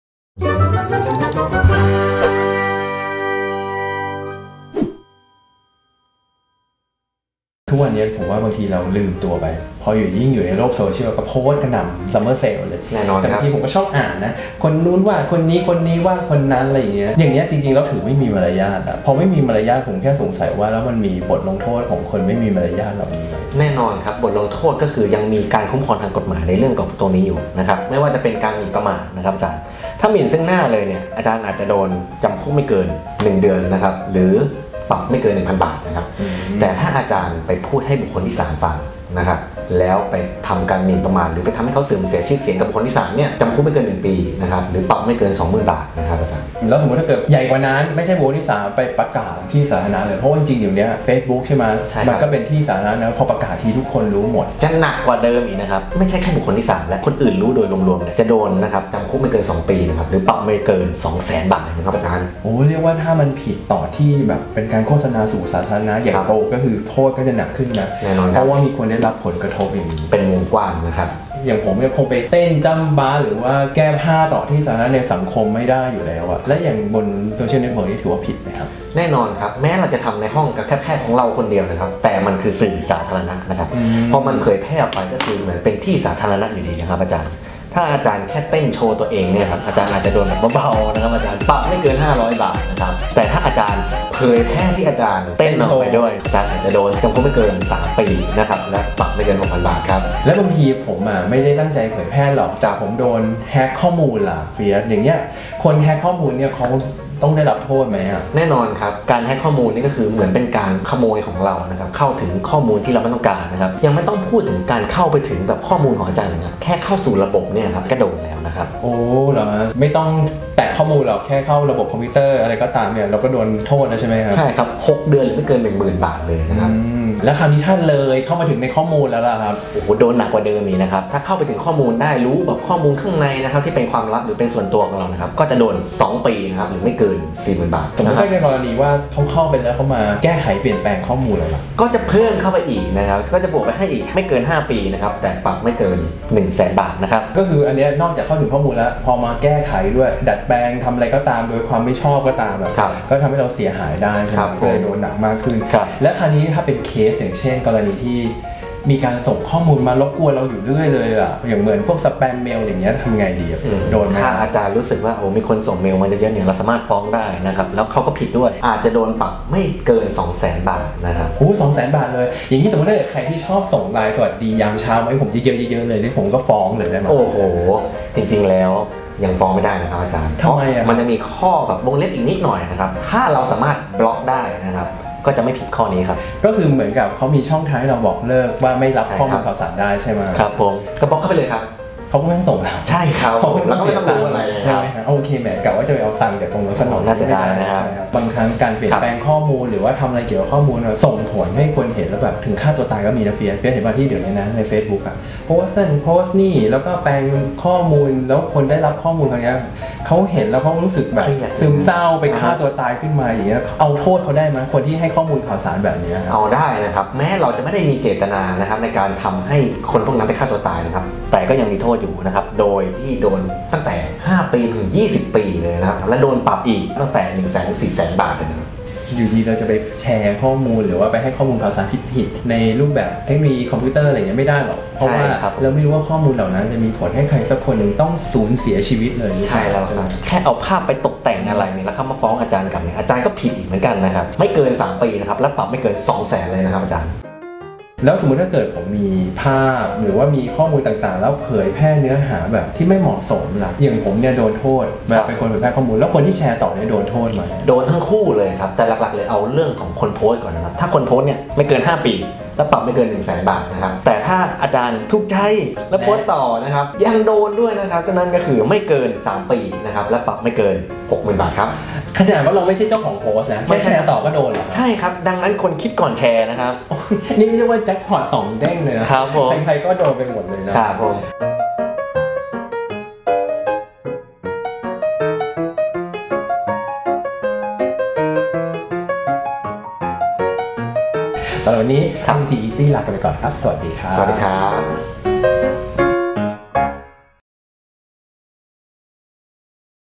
🏄 ICTEZ : ไอซีทีอีซี่ « SE » ตอน โพสต์ แชร์ อย่างไร . . . ให้มีมารยาท
คลิปวิดีโอชุดนี้ เป็นส่วนหนึ่งของโมดูลการเรียนรู้ เรื่อง จริยธรรมในการใช้เทคโนโลยี ในโครงการวิจัยการพัฒนาแหล่งทรัพยากรการเรียนรู้ออนไลน์ในระบบเปิด เพื่อพัฒนาทักษะชีวิตสำหรับเด็กที่มีความบกพร่องทางการได้ยิน โดย รองศาสตราจารย์ ดร.เขมณัฏฐ์ มิ่งศิริธรรม และ อาจารย์ ดร.กรรณ จรรยาวุฒิวรรณ์
▸ STOU CHANNEL [ Educational Channel for All ]